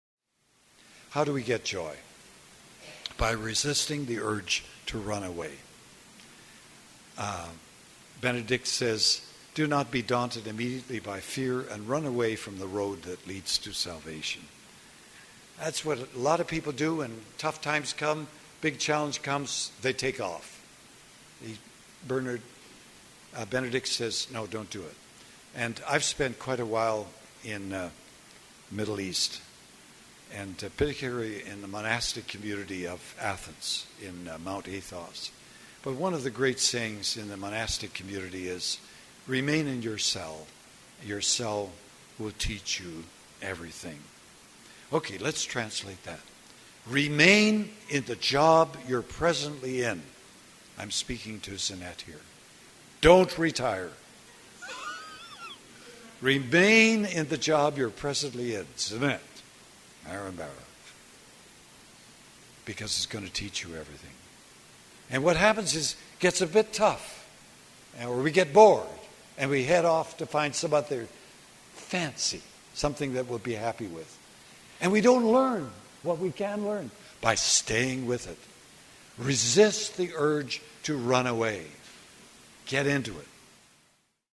Joy@Work 20 How do we get Joy?
Joy @ Work
A Theology of Work Seminar

Speaker : Paul Stevens
Where : Asian Theological Seminary Strategic Leadership Development Center

MBA in Biblical Stewardship and Christian Management

       edit to short clip : Dave Camuyong